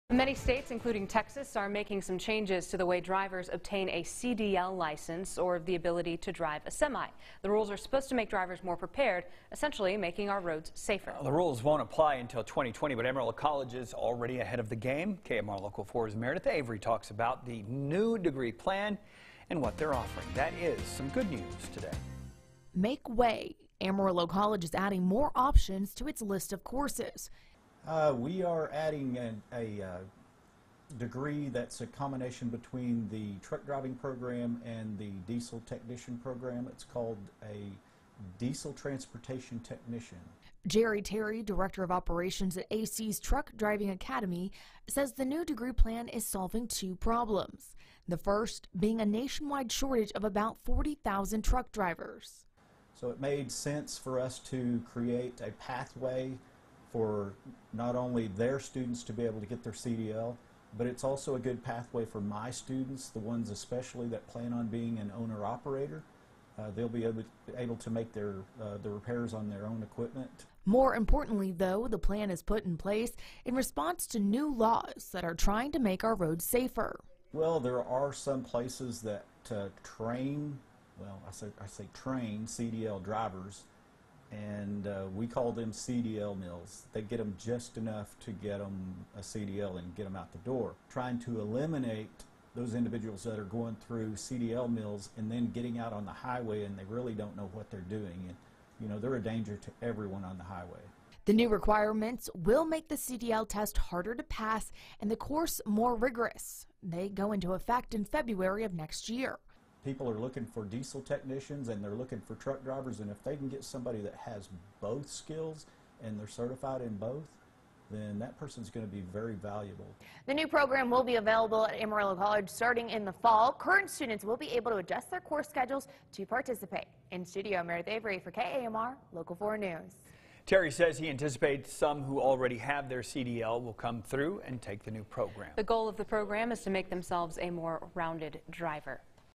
Amarillo College Develops New Program for Truck Drivers and Technicians
Nationwide, states are cracking down on instructors and organizations training drivers for a commercial drivers license. In response to these new regulations, Amarillo College has created a new degree plan, so its’ students will be in compliance upon graduation.